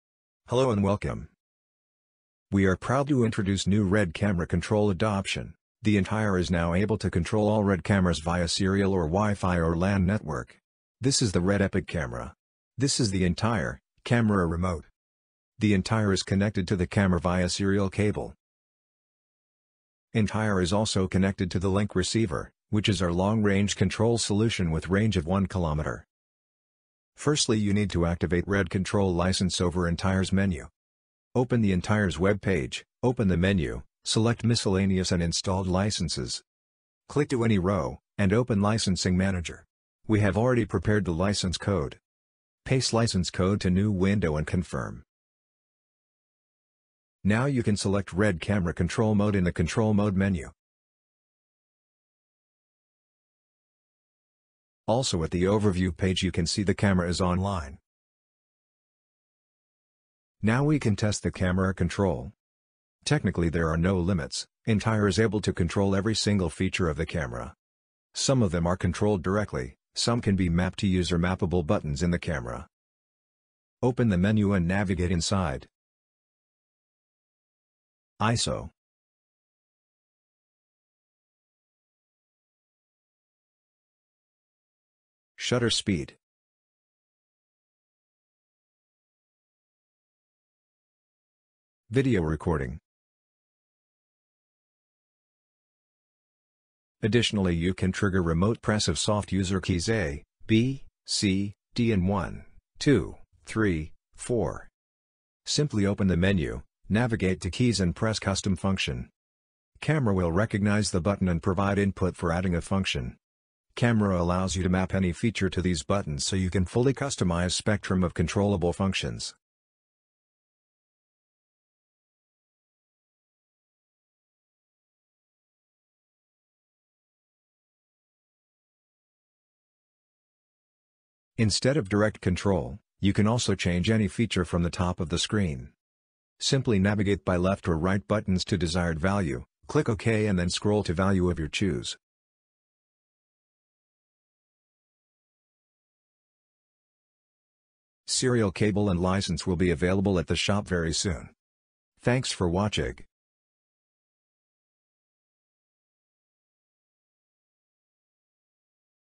RED Cameras long range remote control for drone usage - AIR Commander Link & Entire
This video shows how the Entire can control any RED camera for long range (up to 1km) applications. The Entire can also be used as LAN network control bridge.

The Entire is basically able to control ALL features of any RED camera via CTRL port or via WiFi (suggested).
Directly controlled features:
- Rec start/stop

- ISO
- ShutterSpeed/Angle
- Aperture
- WhiteBalance
- LiveView zoom toggle
- Press of SoftKey A, B, C, D, 1,2 , 3 , 4

You can map ANY feature to SoftKeys in the camera, so technically you can control ALL camera features you would like to.